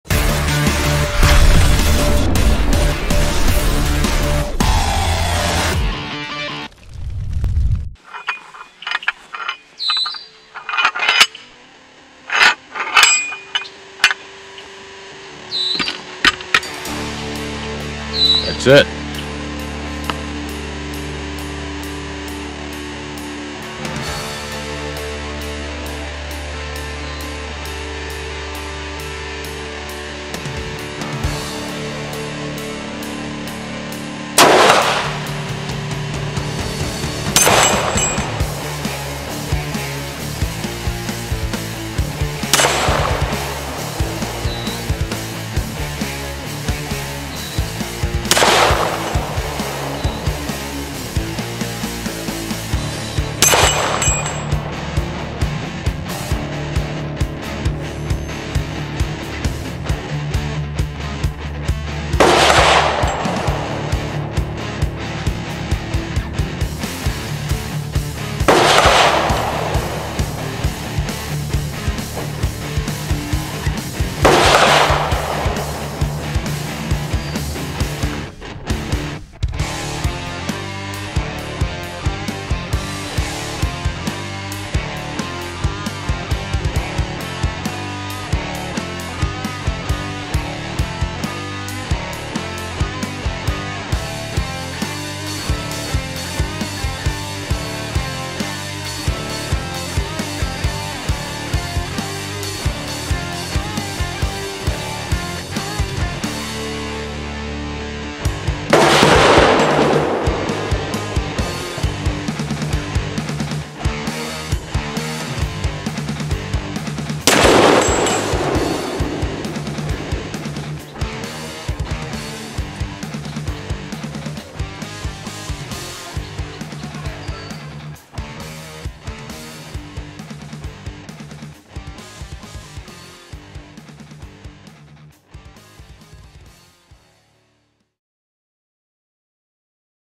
T Box Red Mist Target
The T Box RED MIST Target mounts without tools on a Standard forged T post and uses a self resetting flapper to flash the "red mist"